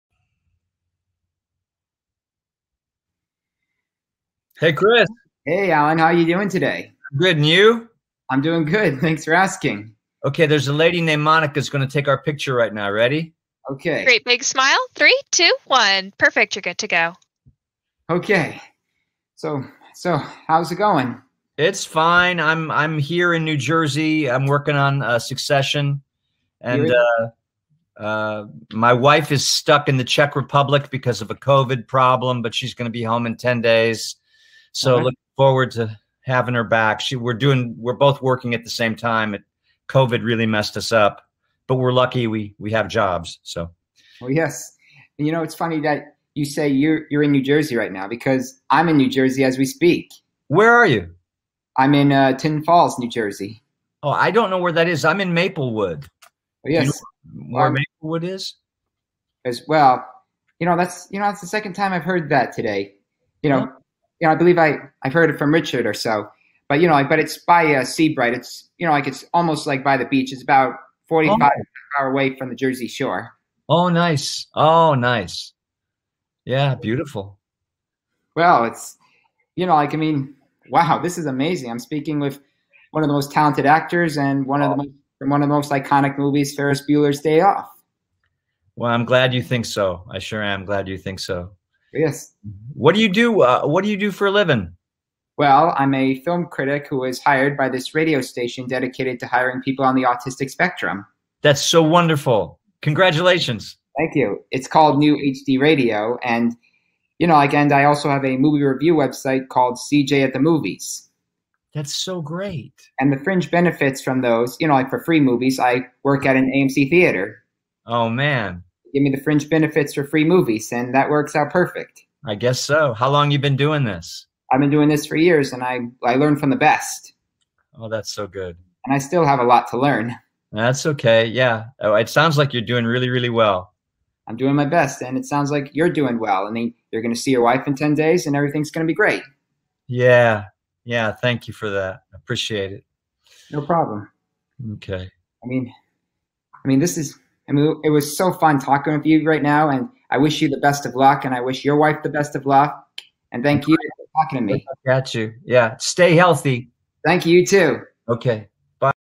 Christopher Zweig's Day Off with Alan Ruck
CJ @ The Movies speaks with the talented Alan Ruck talking about COVID, “Ferris Bueller’s Day Off,” my film criticism, and New Jersey.

 Live from Galaxy Con.